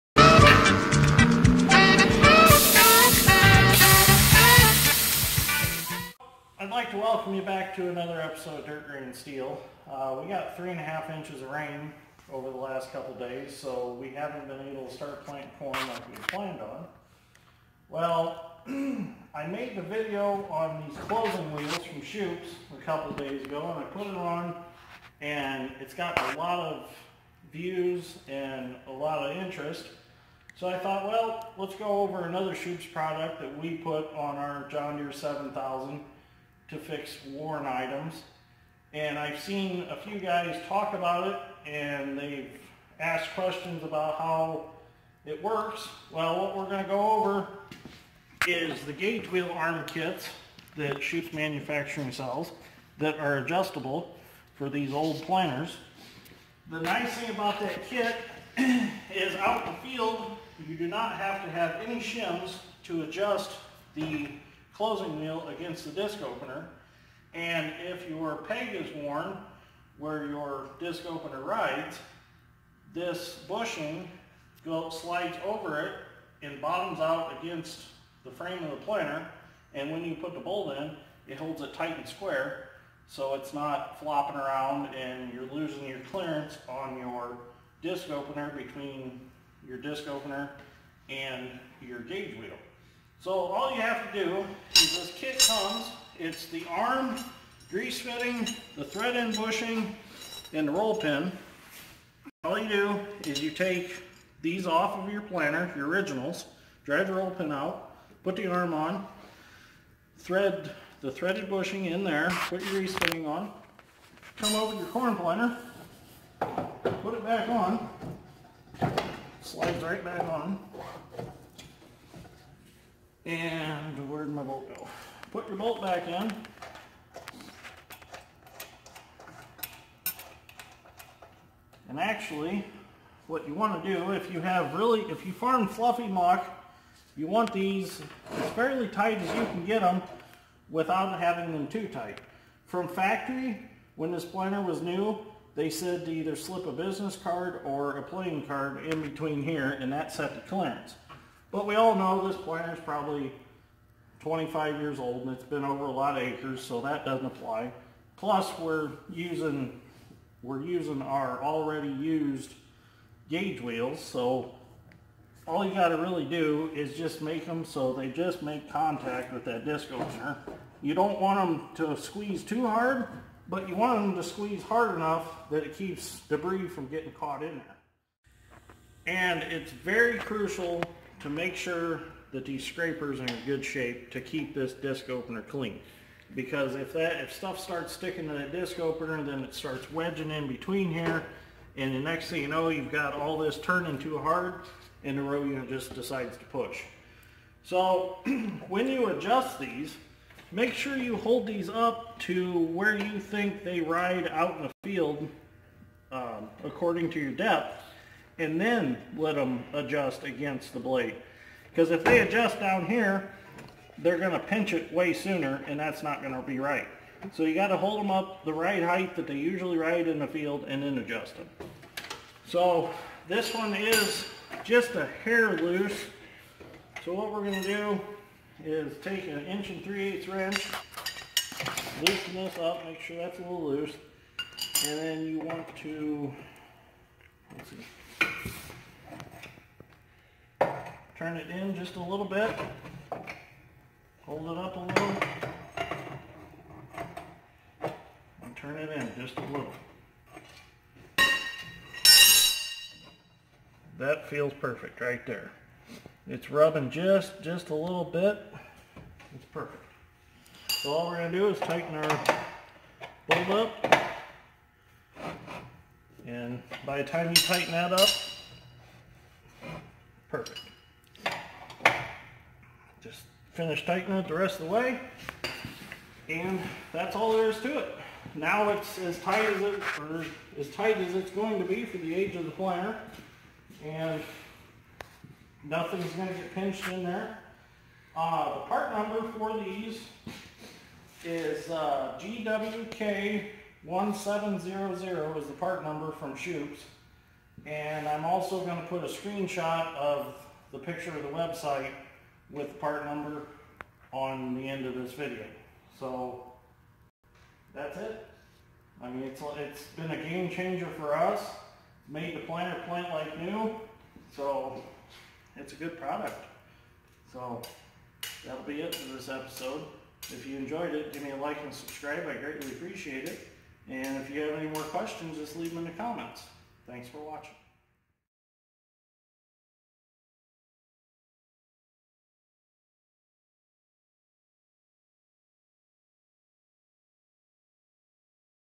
John Deere 7000 corn planter worn gauge wheel remedy!!! With Shoups manufacturing!!
By using Shoups manufacturing's adjustable gauge wheel arm kits we were able to bring our corn back to life and not have to deal with shims and worn gauge wheel shafts this kit is a very easy to install and could be done out in the field